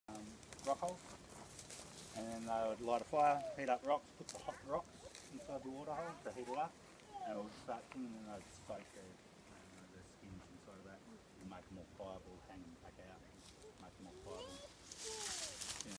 Tanning Hides
Here, Bill is talking about one of the many uses of the balga, otherwise known as 'the Noongar toolbox'.